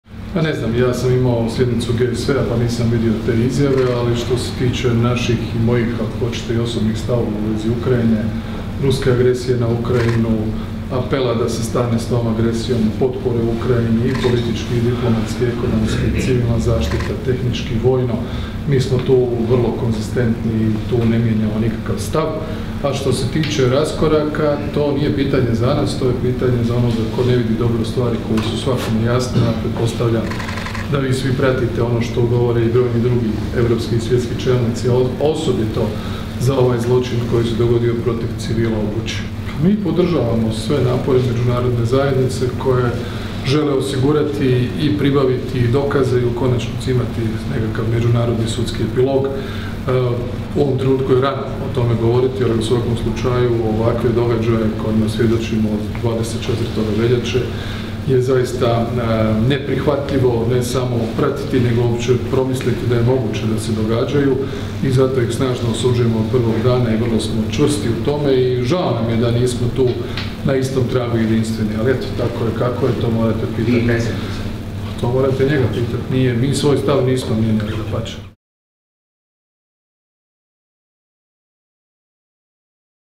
Andrej Plenković o različitim stavovima o Ukrajini Vlade i predsjednika Milanovića
Predsjednik Vlade Republike Hrvatske i predsjednik HDZ-a Andrej Plenković dao je izjavu medijima u središnjici stranke nakon održane sjednice Predsjedništva i Nacionalnog vijeća.